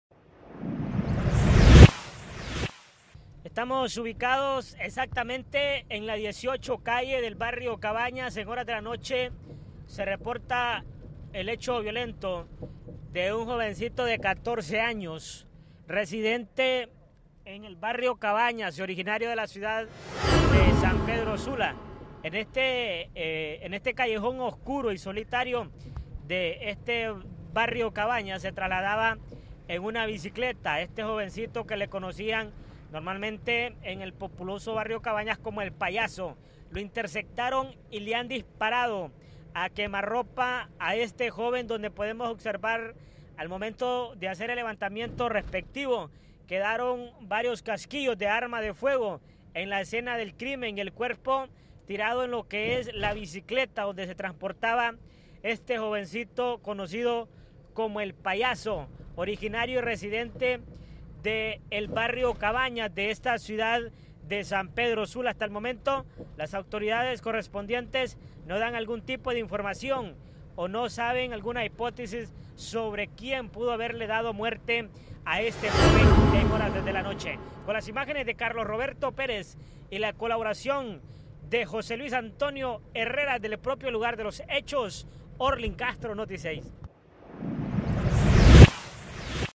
Joven de 14 años fue ultimado en la 18 calle del Barrio Cabañas - Noticias Honduras Canal 6
Noticias Honduras Canal 6

Joven de 14 años fue ultimado en la 18 calle del Barrio Cabañas.

En su dispositivo favorito:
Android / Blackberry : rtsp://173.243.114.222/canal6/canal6